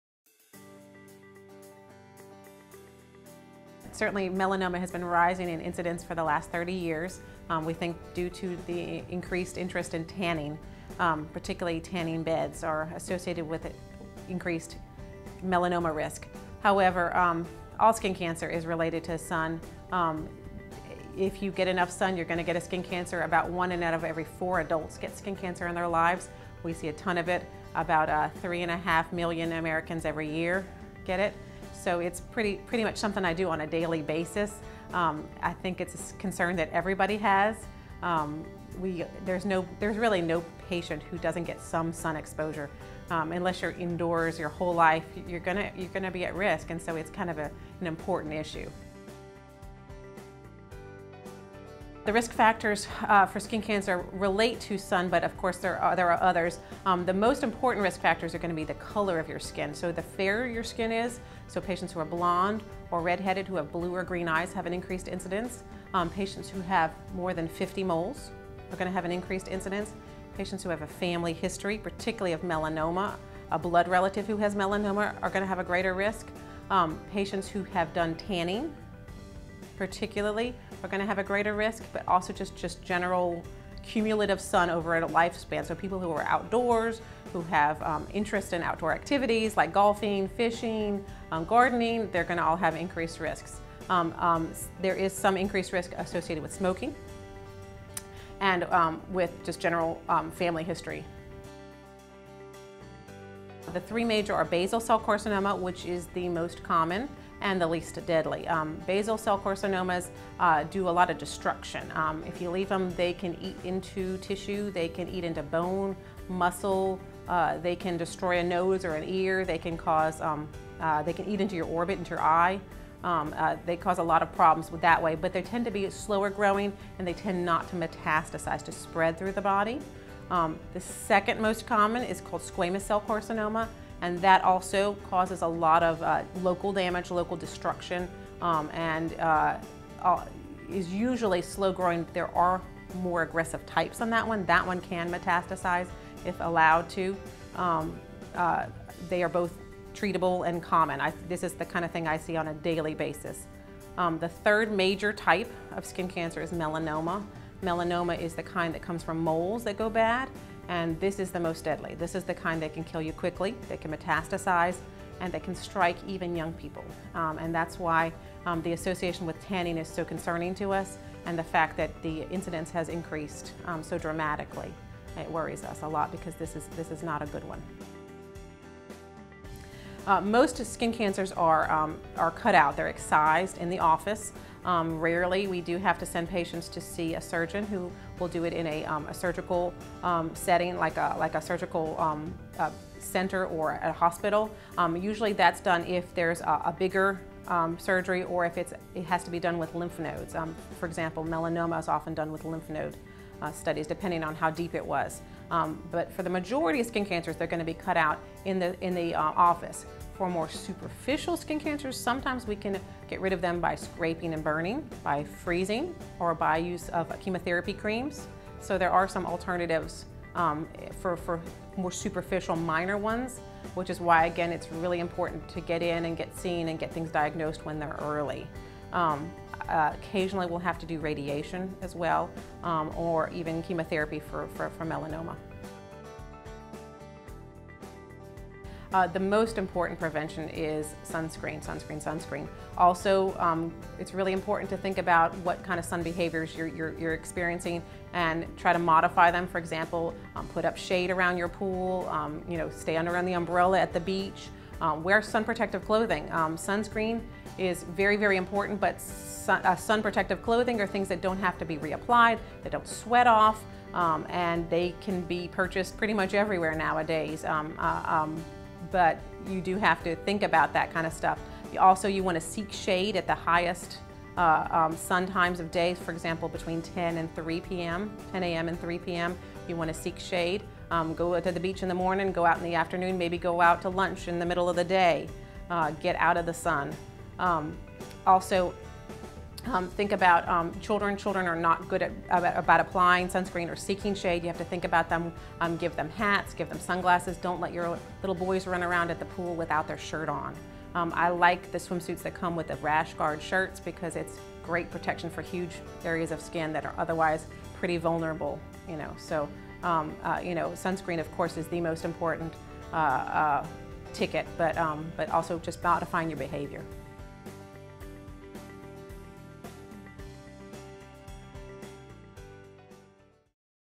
Health Tips: Skin Cancer
Dr. Stephanie Frederic of the Baton Rouge Clinic's Dermatology Department explains how to protect your skin from the sun's UV rays.